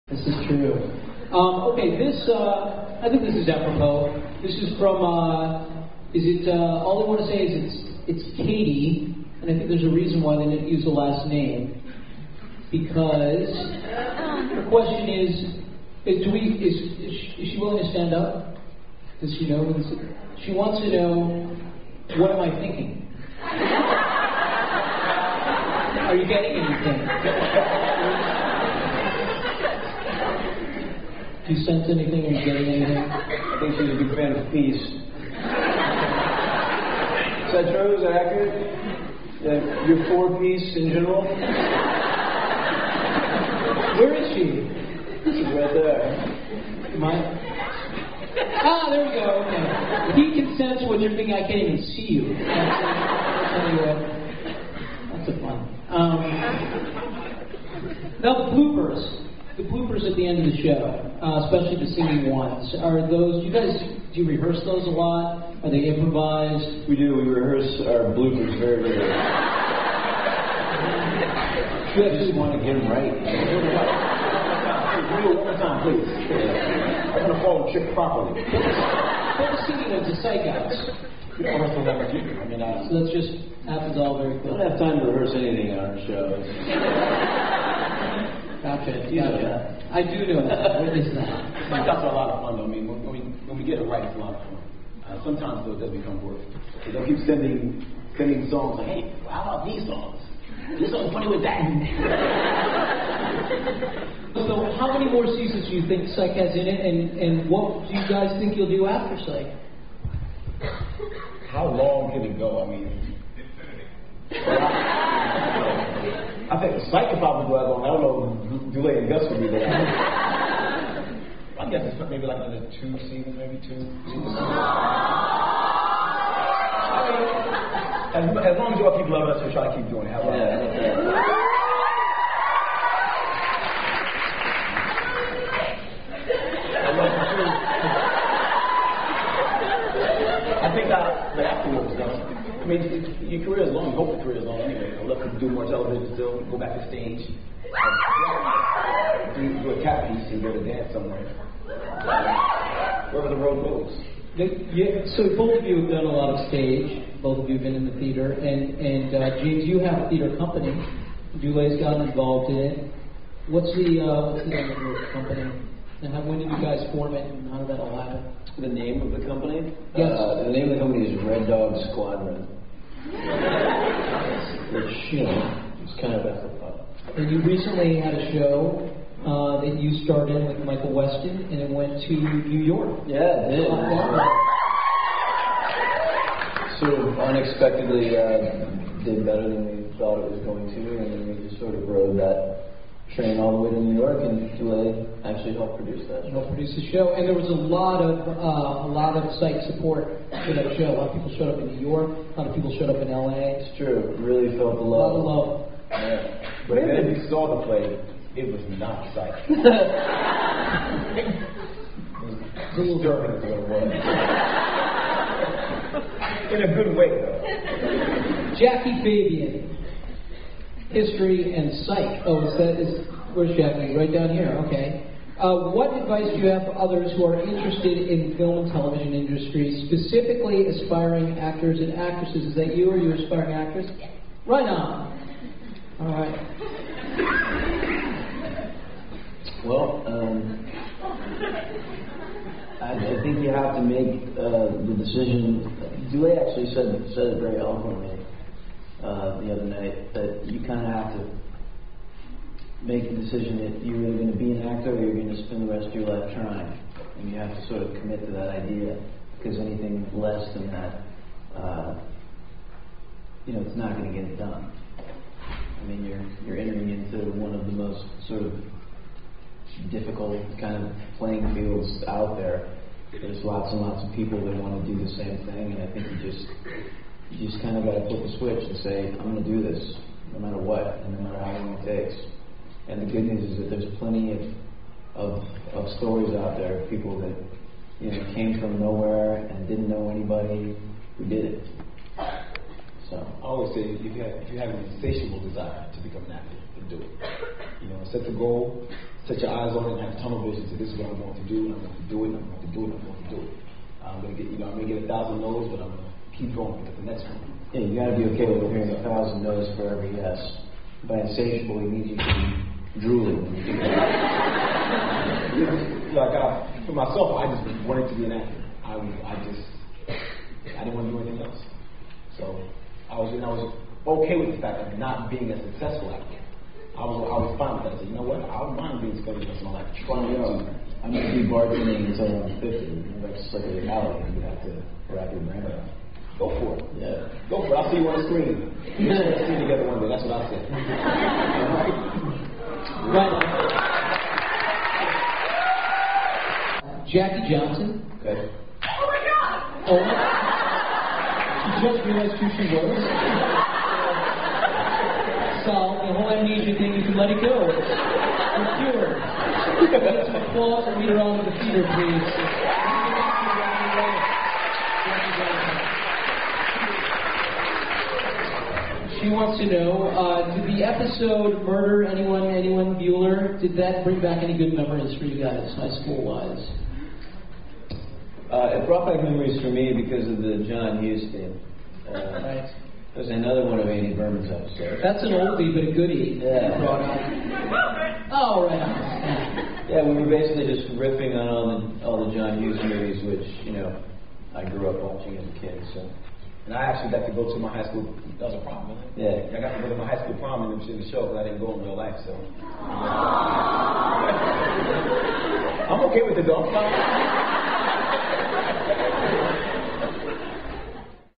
Psych-Part2-James&Dule Q&A
The Psych College Tour At the University of Boston (2010)